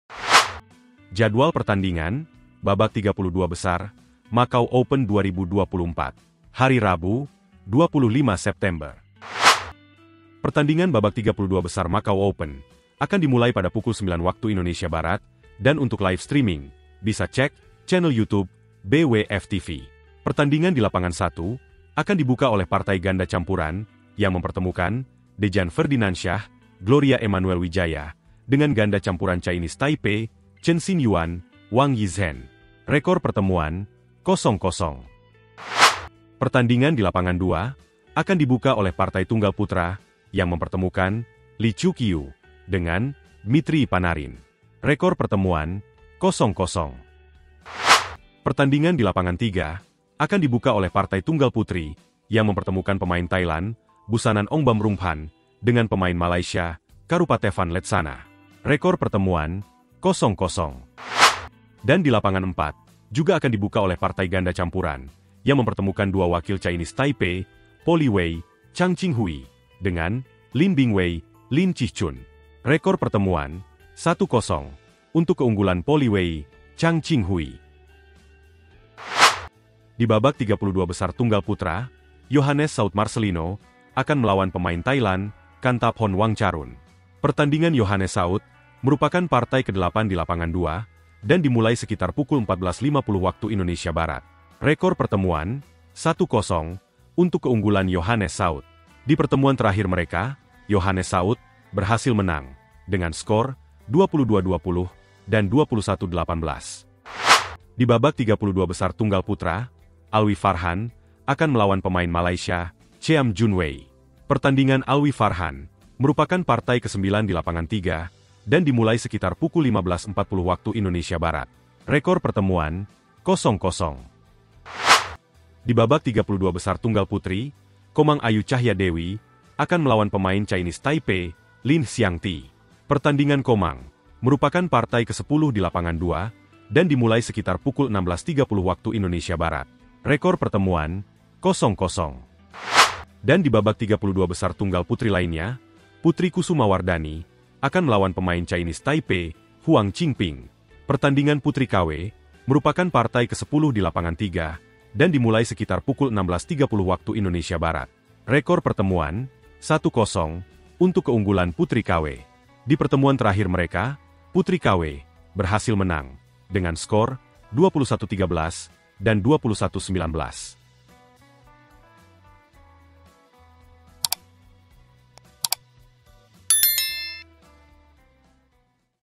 JADWAL BABAK 32 BESAR MACAU OPEN 2024 BESOK PUTRI KW & ALWI MAIN LIVE YOUTUBE BWF TV
JADWAL BABAK 32 BESAR MACAU OPEN 2024 BESOK PUTRI KW & ALWI MAIN MULAI PUKUL 09:00 WIB LIVE YOUTUBE BWF TV